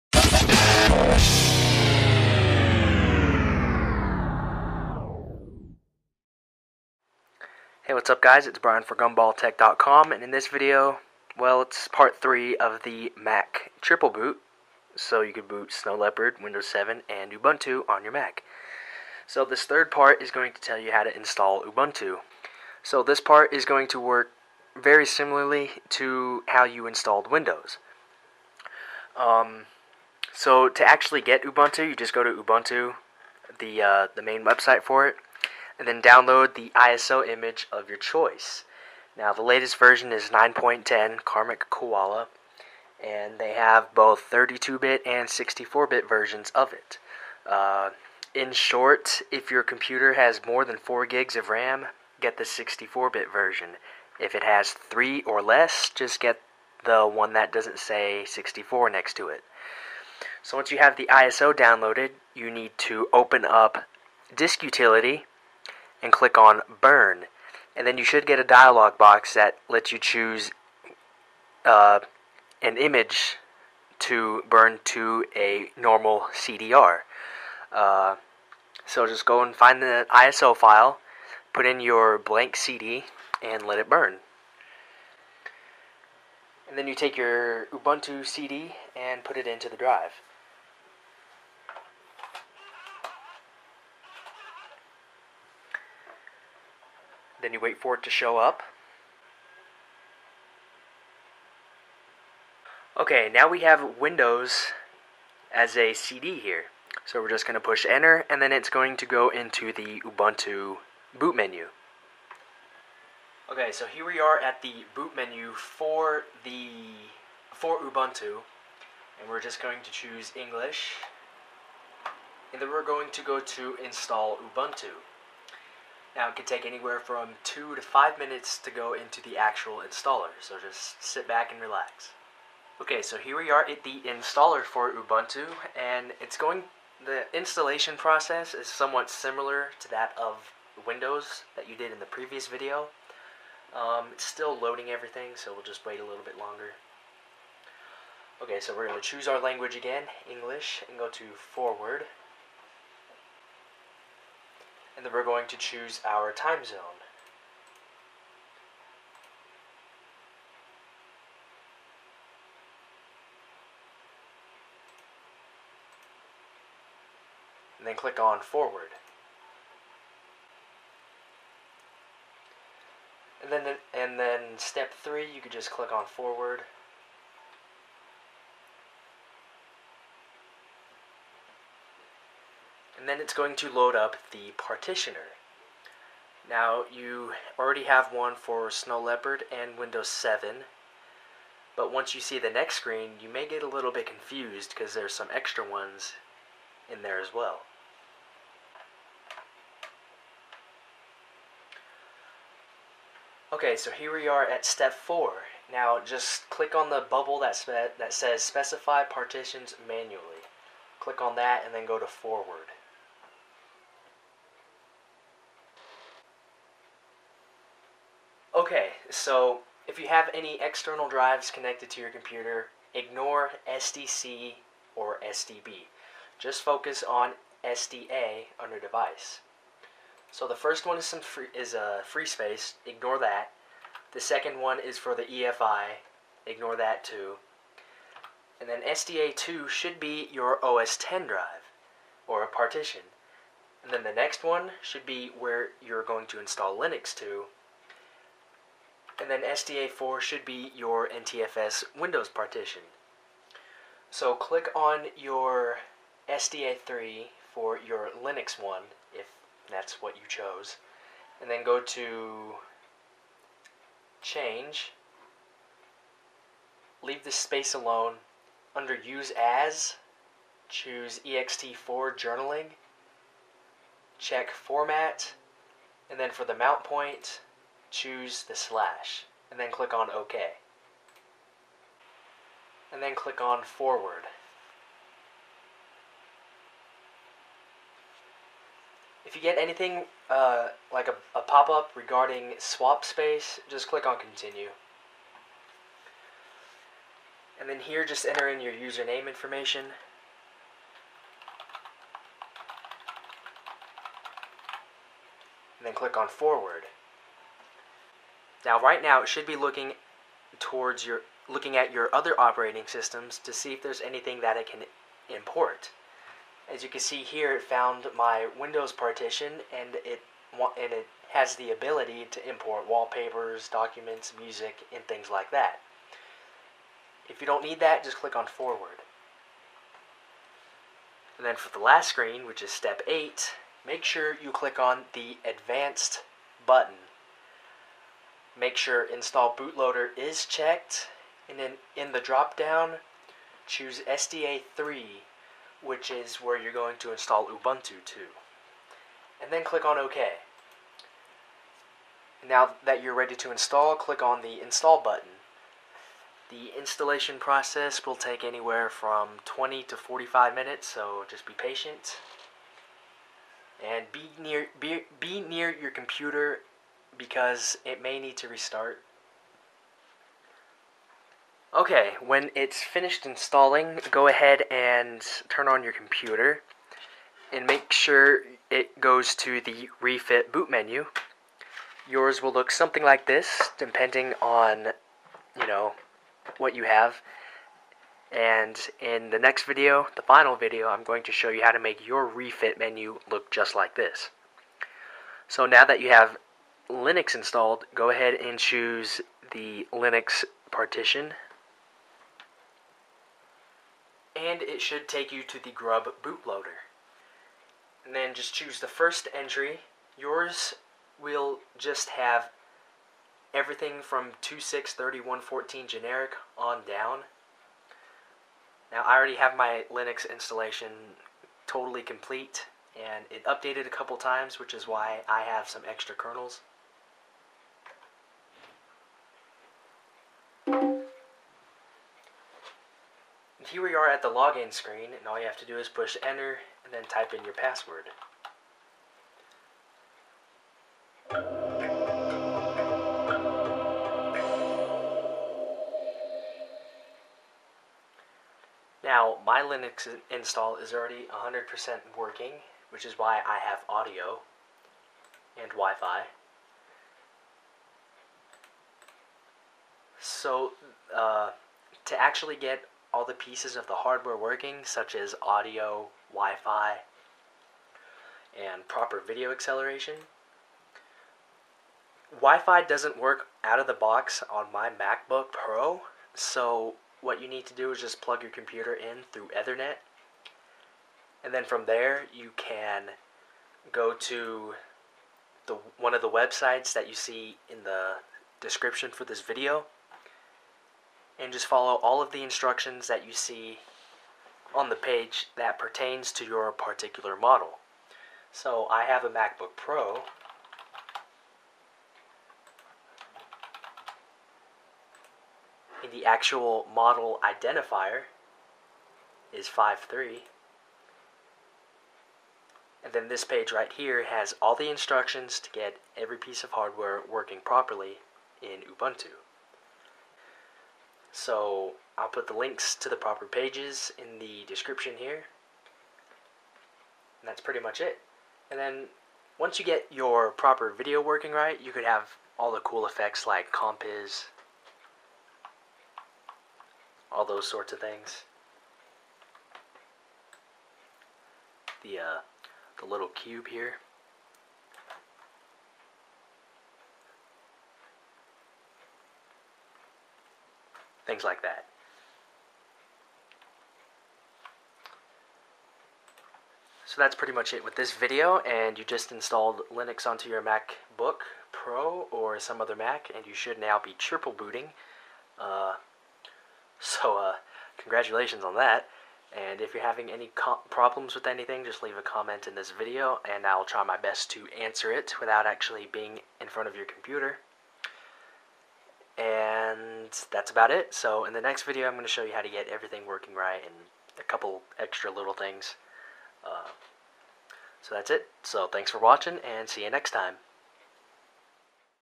[How-To, Pt. 3] Mac Triple-Boot w/OS X+Win7+Ubuntu - Installing Ubuntu
The 3rd of 4 videos showing you how to triple-boot your Mac to make it run Snow Leopard, Windows 7, and Ubuntu.

** ALWAYS MAKE BACKUPS OF YOUR FILES BEFOREHAND! **

The third part of the series tells you how to install Ubuntu:
1) Install Ubuntu - Hold ALT/option when turning on your Mac, and put in the installation DVD. Double-click on "Windows". Follow the rest of the video.
2) Install the proper drivers to make everything work properly - Run the following command in a Terminal window in Ubuntu:
sudo dmidecode -s system-product-name
Once you find the model of your Mac, go to the following link, find your Mac, and follow all the instructions to get everything working properly

The next video will show you how to make your rEFIt menu look like mine.